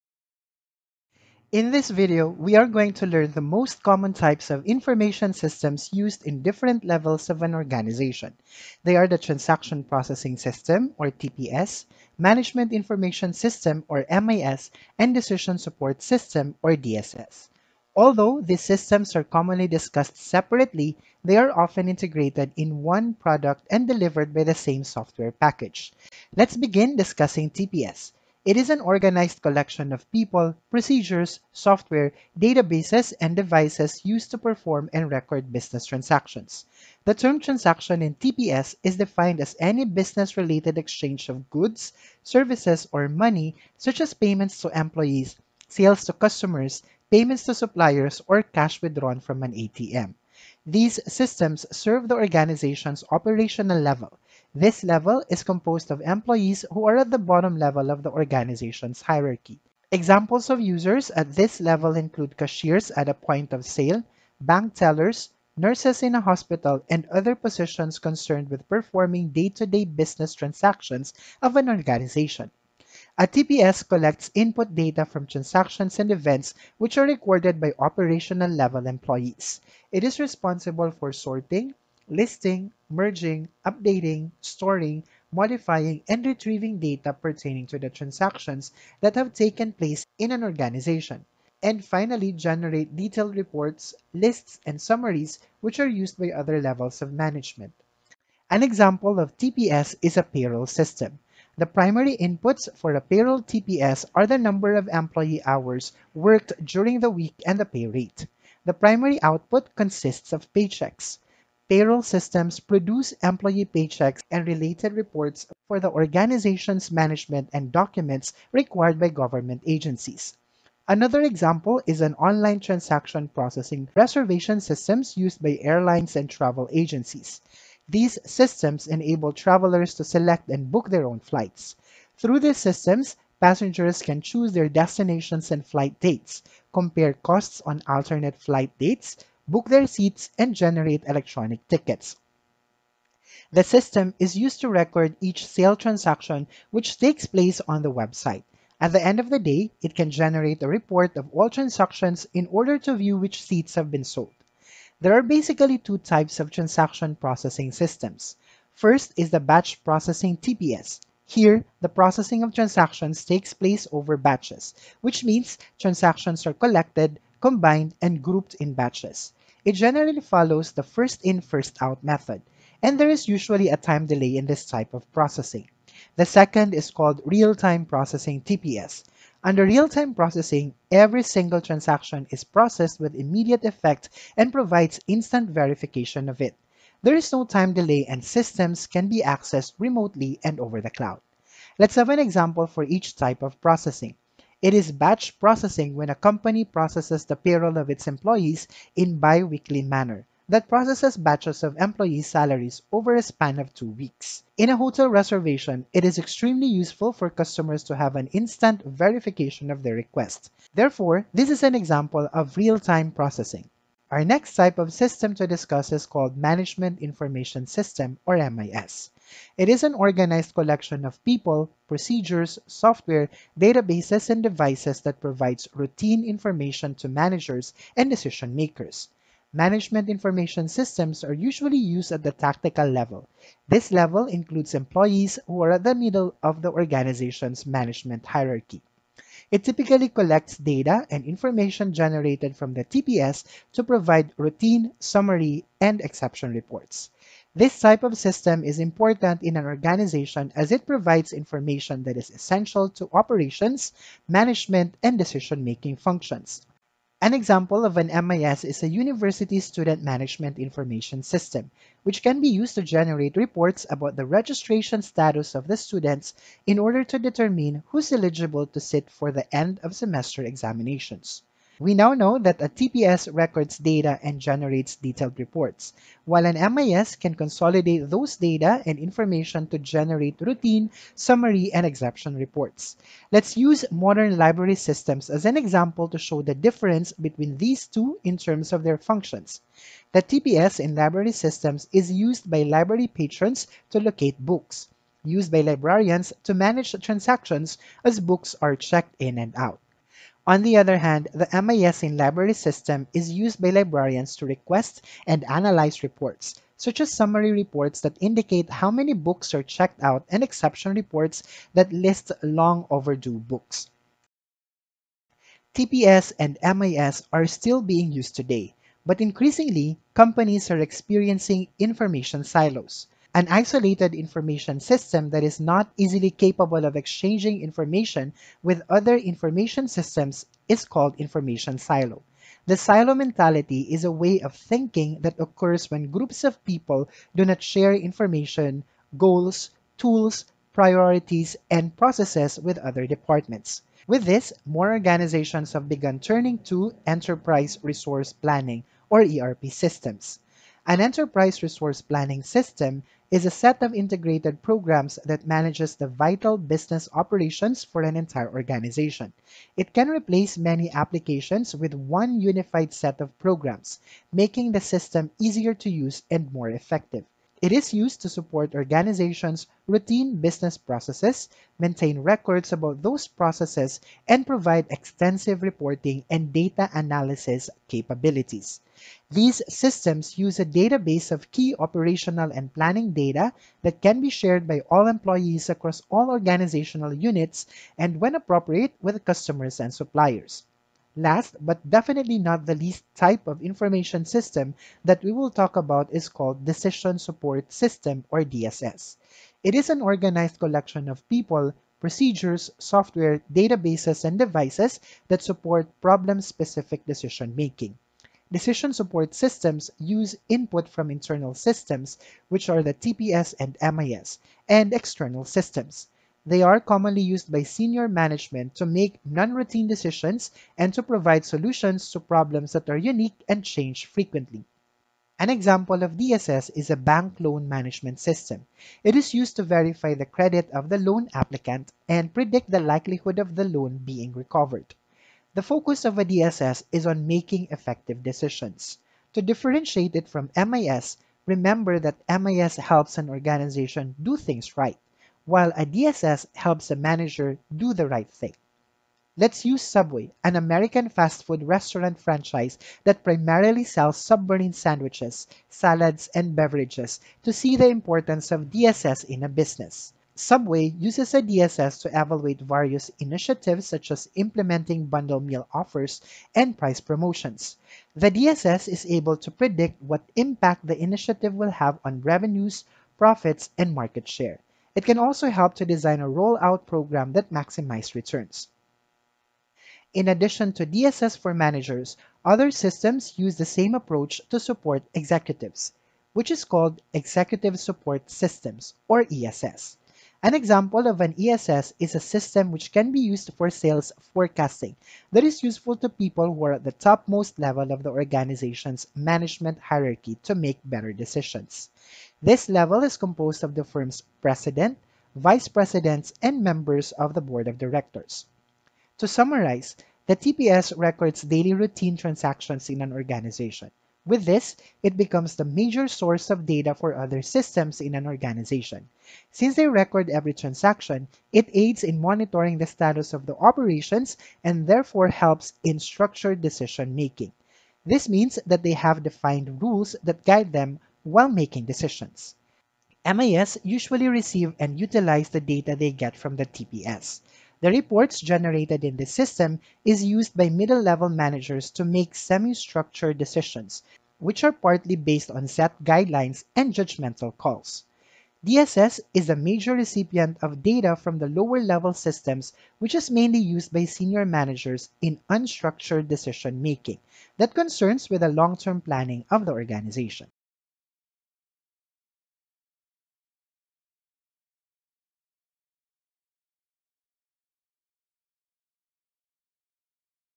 Types of Information Systems (TPS, MIS, and DSS)
In this video, we will learn the most common types of information systems used in different levels of an organization, which are as follows:
1. Transaction Processing Systems (TPS);
2. Management Information Systems (MIS); and
3. Decision Support Systems (DSS).

By the end of the video, learners will be able to
1. Identify the information systems used in each level of management hierarchy in an organization;
2. Describe key characteristics and examples of TPS, MIS, and DSS;
3. Differentiate batch processing and real-time processing;
4. Discuss the term information silo; and
5. Compare TPS, MIS, and DSS.

References:
📔 Stair, R. M., & Reynolds, G. W. (2016). Fundamentals of information systems (Eighth Edition ed.). CENGAGE Learning.
🔗 (n.d.). Types of information system: TPS, DSS & Pyramid Diagram.
🔗 MBAGROUP1BLOG. (2021, July 14). Question 2: describe the relationship between TPS, MIS and ESS. Describe each with examples from a real Kenyan or African scenario.
🔗 Naini, A. (2021, June 4). What is transaction processing system?

🔖 Thanks to the Flaticon Google Slides add-on for the free icons.

00:00 Introduction to Types of Information Systems (IS)
00:26 Transaction Processing System (TPS)
02:36 Types of TPS
03:34 Management Information System (MIS)
04:34 Difference of TPS and MIS
05:23 What is Information Silo?
05:51 Enterprise Resource Planning (ERP) System
06:31 Decision Support System (DSS)
07:05 MIS vs DSS
07:30 Importance of DSS to Businesses
08:06 Executive Support System (ESS)
08:30 Comparison of TPS, MIS, and DSS
09:21 References
09:24 Subscribe and More Videos